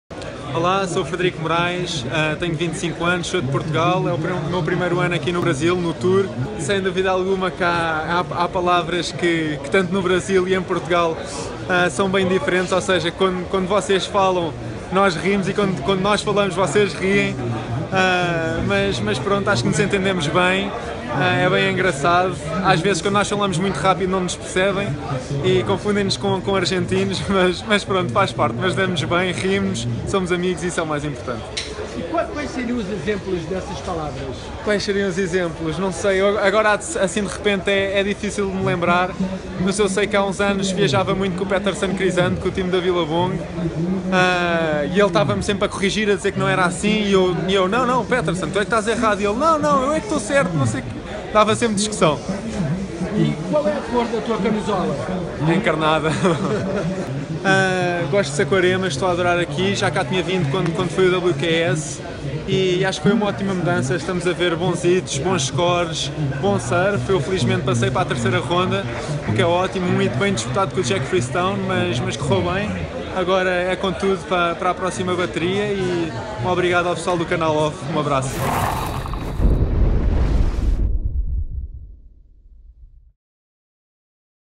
Frederico Morais - Round 2 | RioPro2017 | Circuito Mundial de Surfe | Saquarema | Canal Off
Frederico Morais venceu a sua bateria no round 2 e se prepara para enfrentar o Australiano Julian Wilson na próxima faze. Confira uma entrevista descontraída com o atleta.